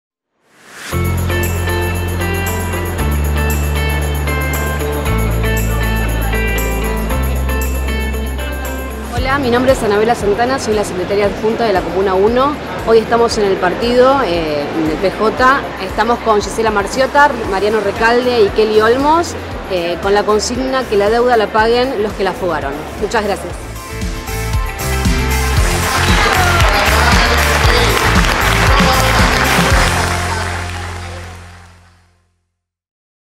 “Que la deuda la paguen los que la fugaron”
Reunión en el PJ junto a Mariano Recaldo, Gisela Marziotta y Kelly Olmos bajo la consigna: “Que la deuda la paguen los que la fugaron.“